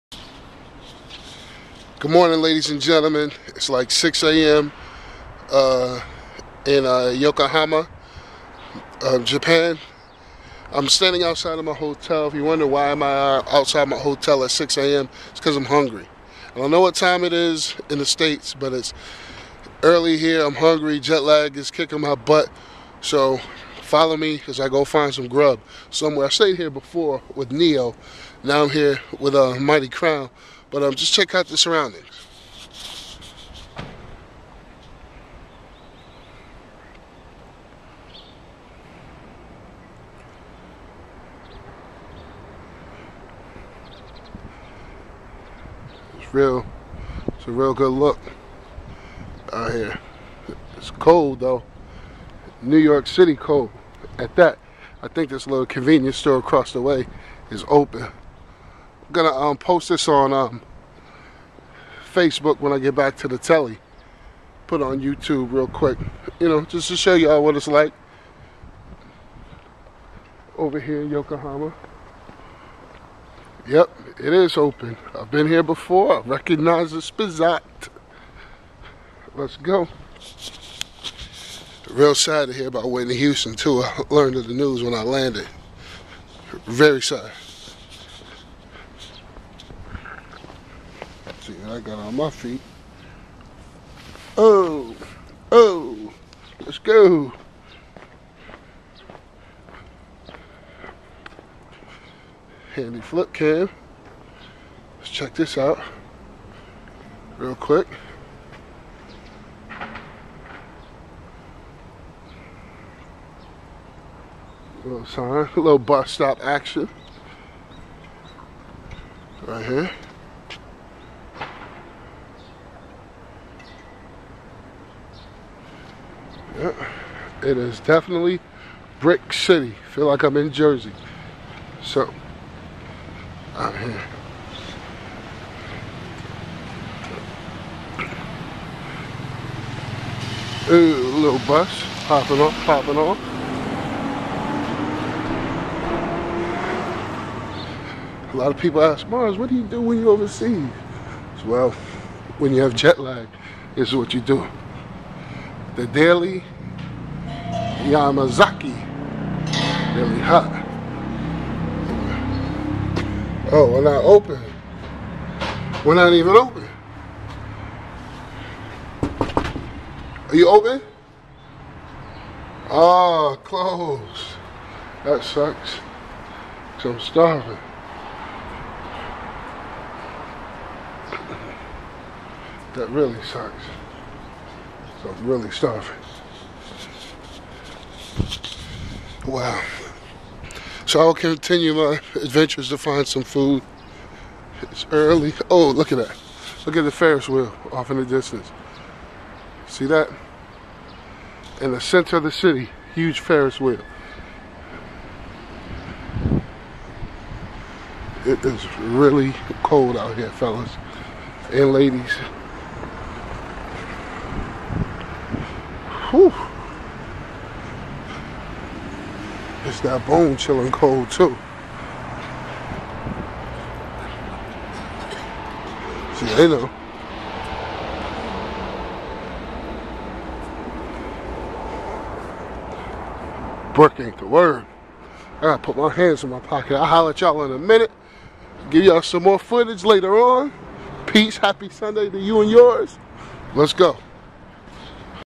DJ MARS EARLY MORNING BREAKFAST WALK IN YOKOHAMA JAPAN
So im up very early in Japan,starving to say the least. Peep the freezing cold and very brisk walk on my hunt to find food.